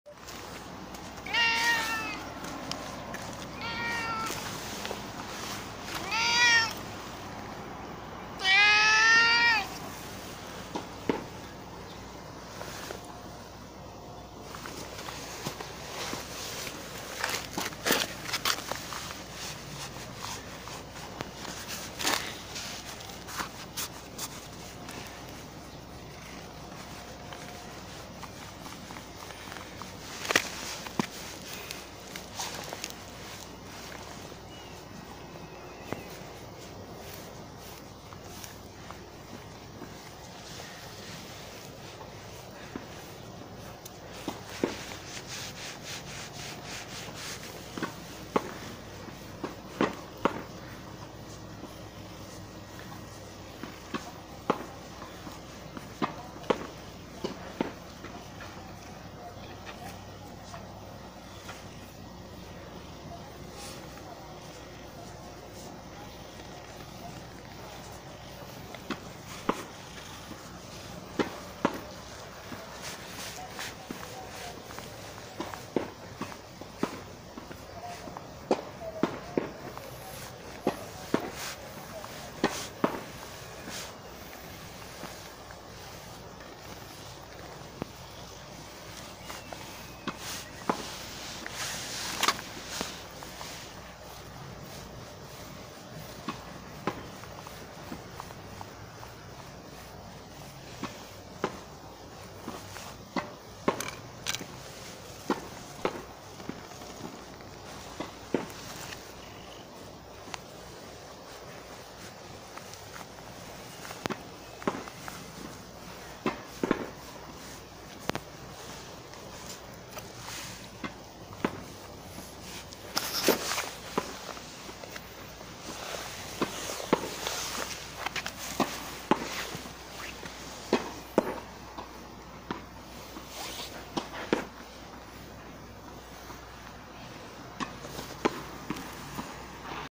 強そうだけど甘えん坊な茶トラ野良猫
人相（猫相?）悪い野良猫ちゃん、顔や背中は傷だらけでケンカの跡なのか?でも見かけによらず甘えん坊。ゴロンゴロン転がってお腹をモフモフ要求してきた。

怖い！お腹に乗ってた猫が急に稲川淳二が怪談話する顔になる　何を見たのか